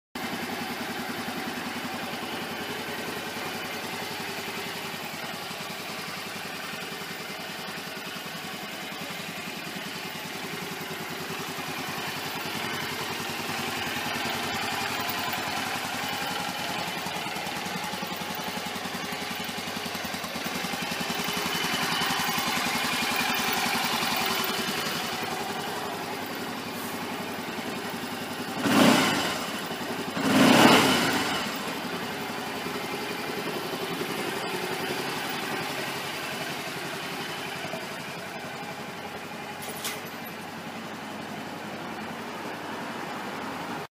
エンジン始動/マフラー音 スカイウェイブ250 Limited 2006年 中古車 バイクショップ名:SURFACE (サーフェイス)
ビッグスクーター専門大型店舗 ★サーフェイス★ JR環状線寺田町駅すぐ♪
整備後のお渡しで安心の保証付きです
わからない点や気になる点は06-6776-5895までお気軽に